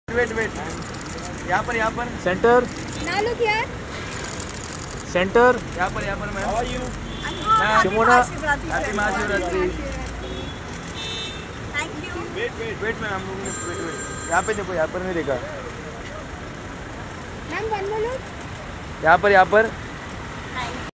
Kapil Ki Sarla Sumona Chakravarti Snapp in Mumbai
Kapil Sharma Show Fame "Sarla" Actress Sumona Chakravarti Snapp in Mumbai